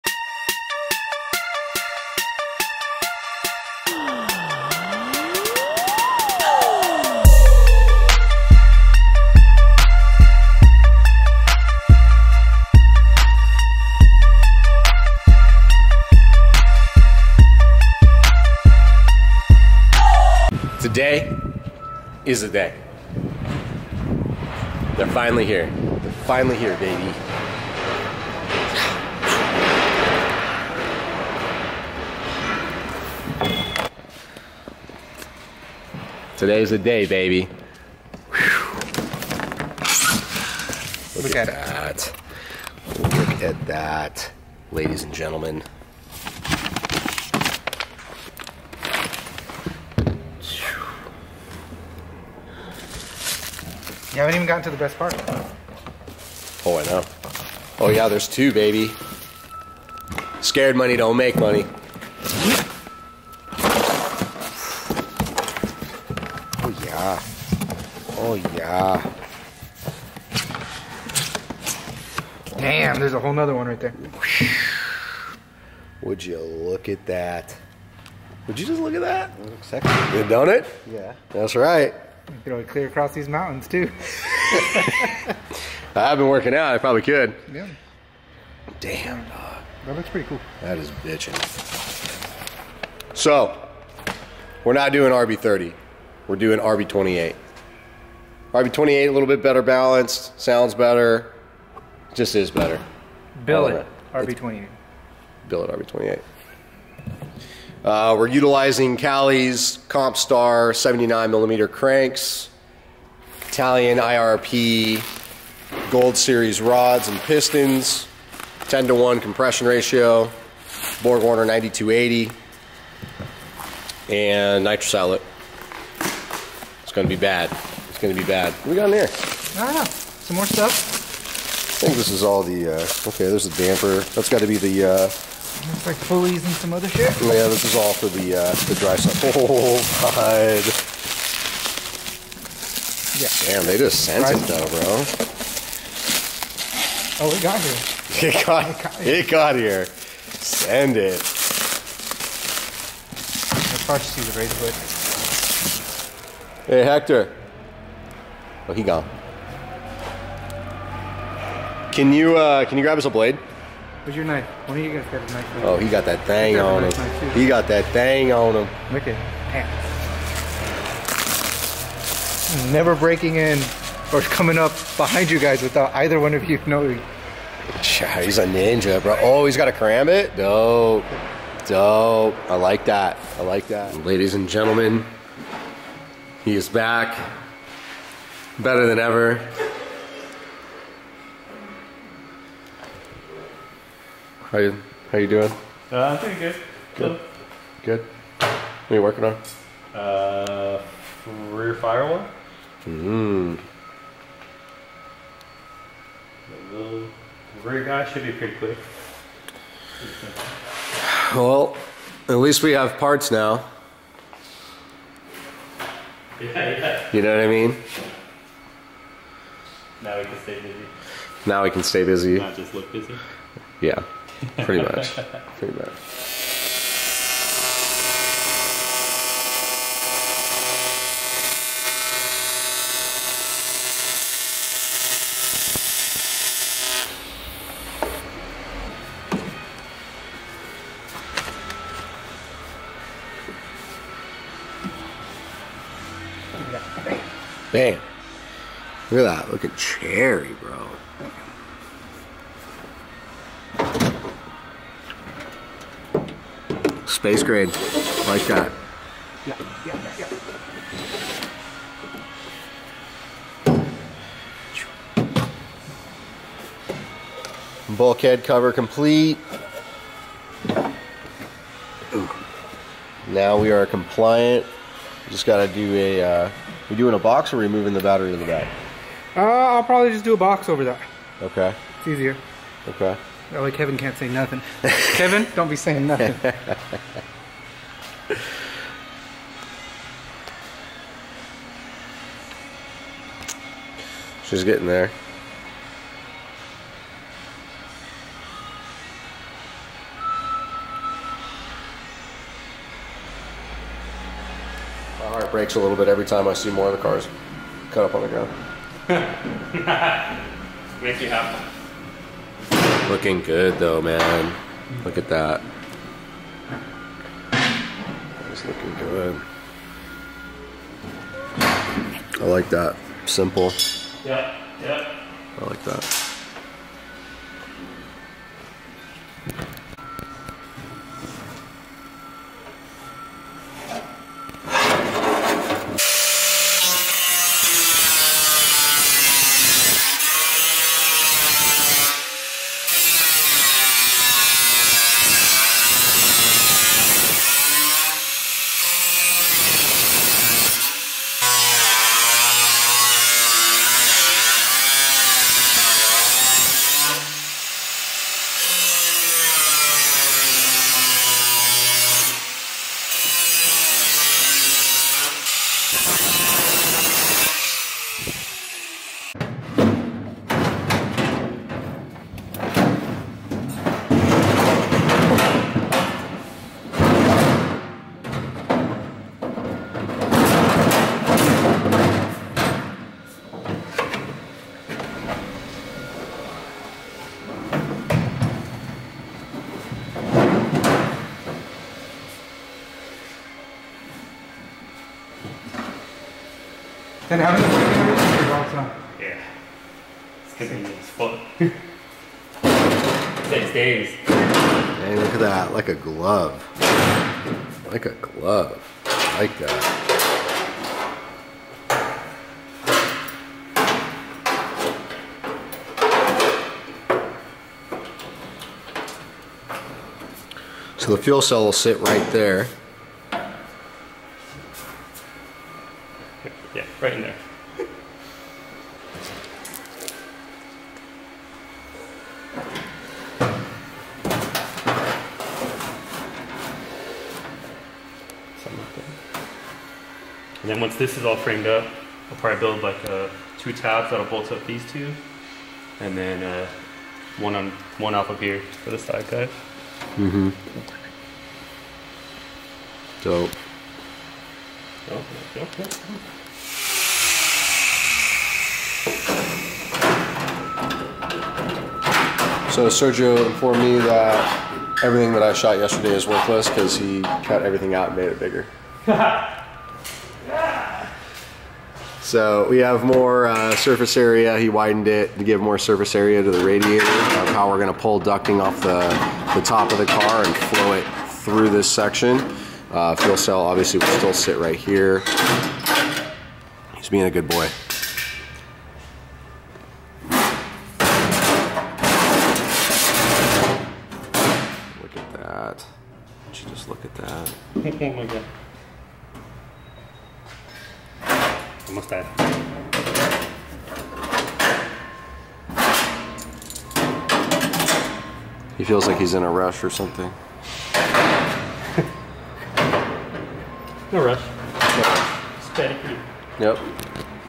UNBOXING THE BILLET BLOCKS! LETS GOO!!!! - Dream S15
YOOO! Today is THE day! Unboxing the beautiful Bullet Race Engineering billet RB26 blocks for our 2.8 stroker engines that we will be running this year! Stoked to have these guys on my side and this will be a game changer for RB is professional drifting competition. 4 stage dry sump, Borg Warner 9280 EFR turbocharger, Callies 79mm billet crankshaft- these engines will be badass!